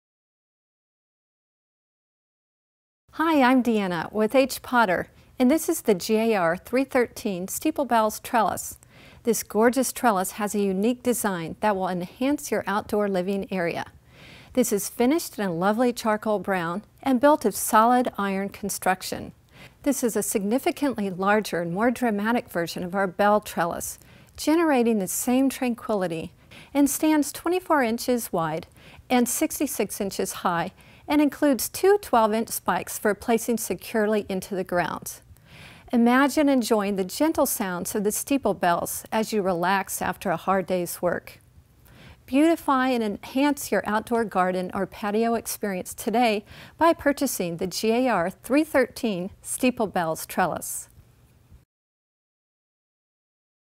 Trellis
A significantly larger and more dramatic version of our Bell Trellis, the Steeple Bells Trellis generates the same tranquility when the bells become wind chimes in the breeze. The unique charm of this yard trellis is complemented by climbing flowers, but there's no denying its perfection all on its own.

Charcoal brown powder-coated finish

24"W x 66"H (includes two 12-inch spikes)